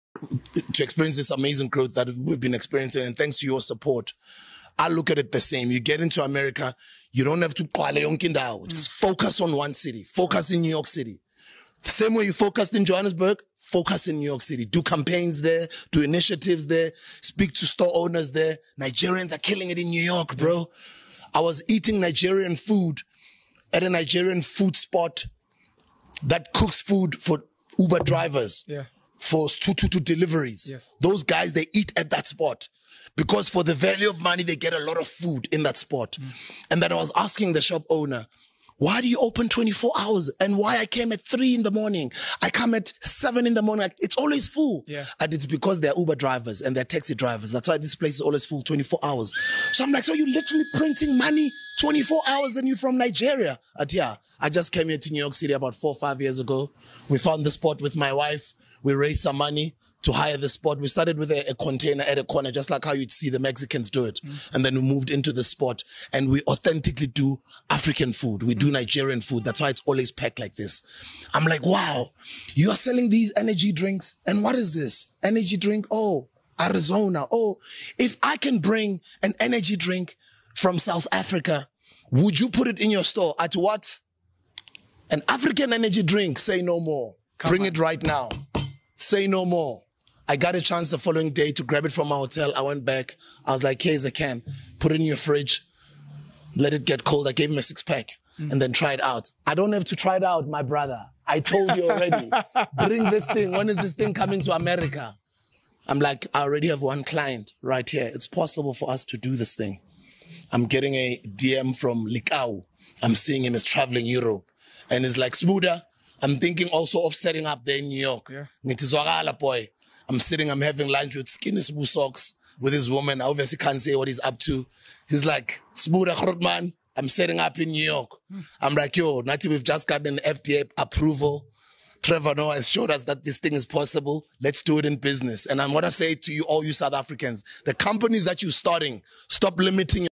to expand MOFAYA to America
Dj Sbu has been FDA approved to expand MOFAYA energy drink in the United States of America